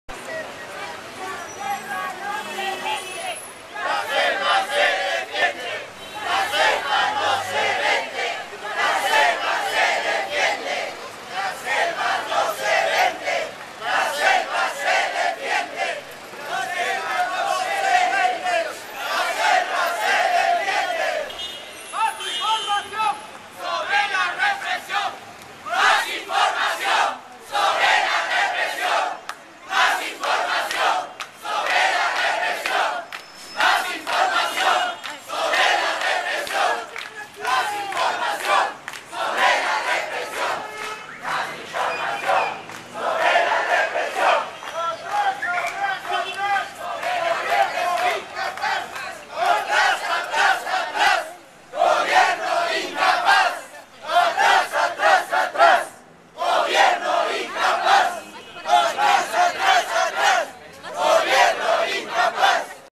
Marcha frente a canal 5
Manifestantes marchan frente a canal 5 exigiendo mayor difusión